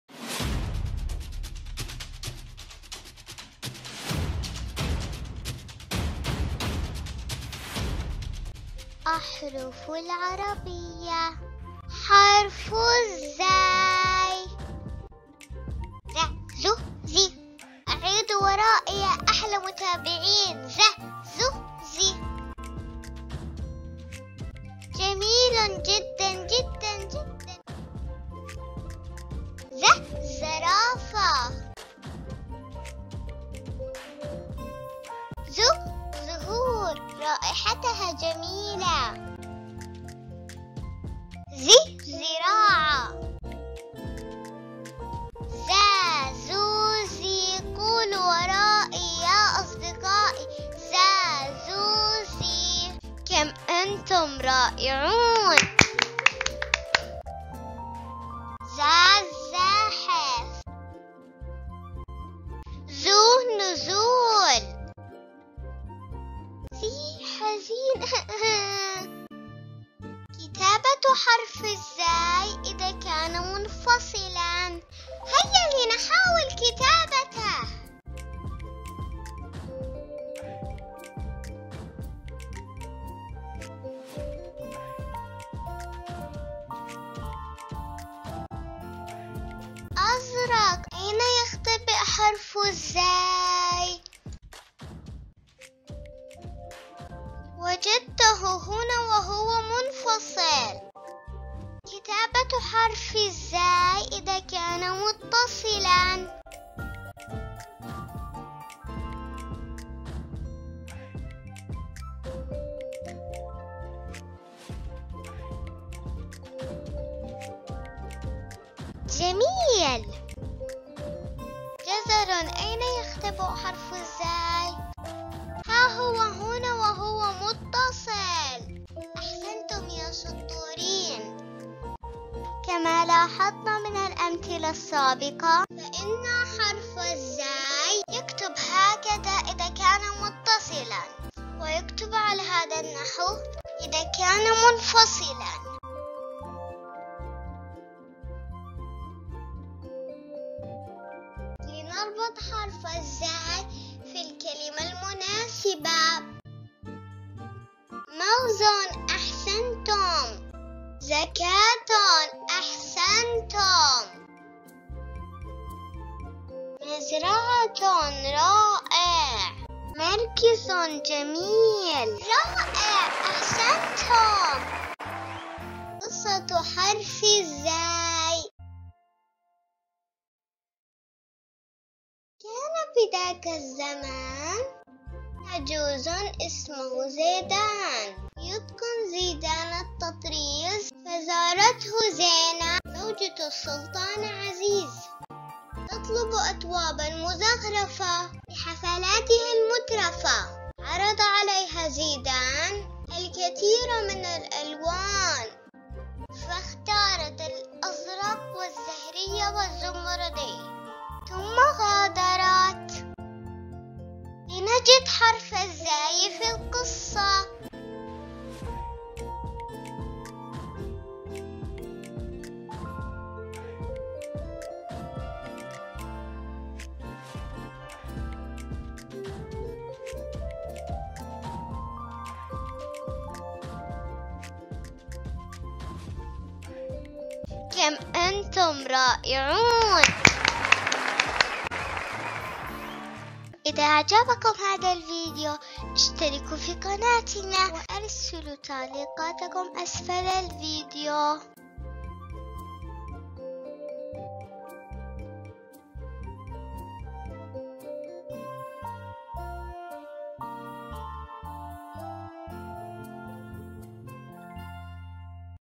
alphabet_learning: the Arabic Alphabet with Us! حرف ز
أحرف العربية ، أهلاً بكم في حرف الزاي
 حرف ز:
 الزاي المتحركة: زَ زُ زِ
الزاي الممدودة: زا زو زي
كيف نكتب حرف الزاي إذا كان متصلاً و إذا كان منفصلاً .
أمثلة على كلمات تحوي حرف الزاي مع صور رائعة.
أين يختبئ حرف الزاي؟
قصة حرف الزاي
ساعدوني لنجد حرف الزاي في القصة

تعلموا معنا الحروف الأبجدية
 |  قناة وفرة  | Wafra tv
قناة وفرة للمحتوى الأكثر فائدة للأطفال 🔔👍🏻

Learn the Arabic Alphabet with wafratv
   حروف الهجاء - تعليم الأحرف العربية للأطفال